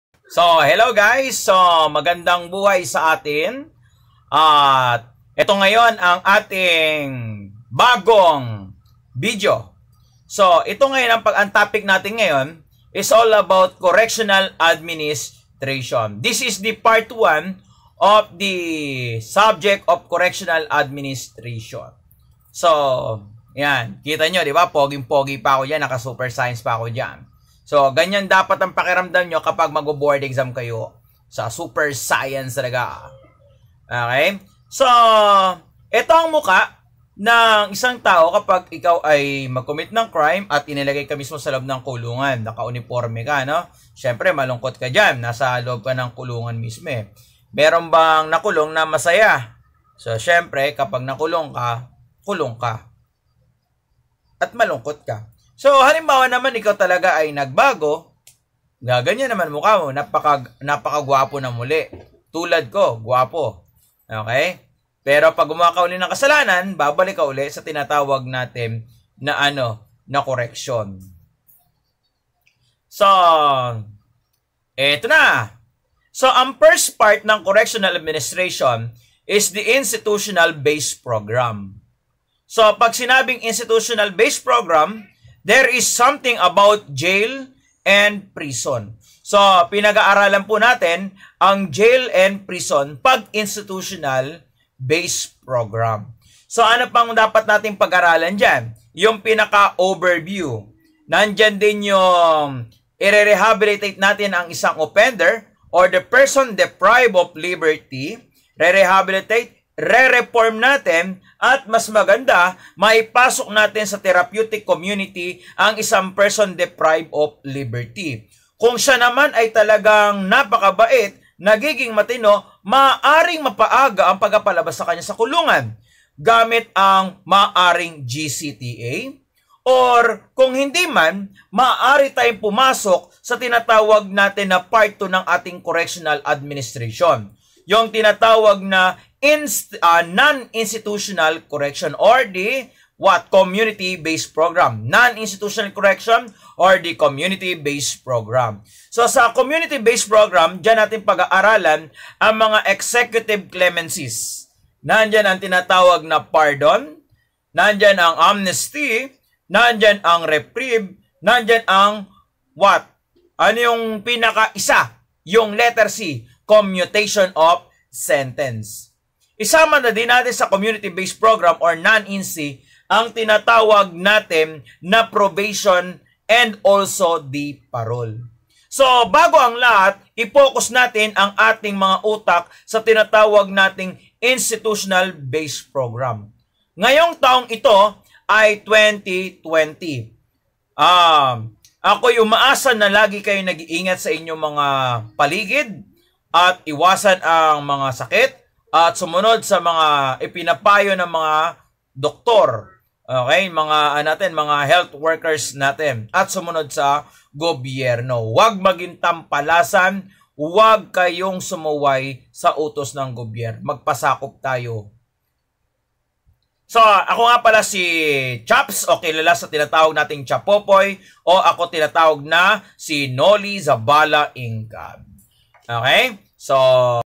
Institutional Correction
ACLE Mentoring by Chaps
Baby gusto mo CONJUGAL VISITATION?

Online review is real, kasama ang Mastering Criminology Terms w/ q and a 2020.

Introduction to Institutional Correction (Overview)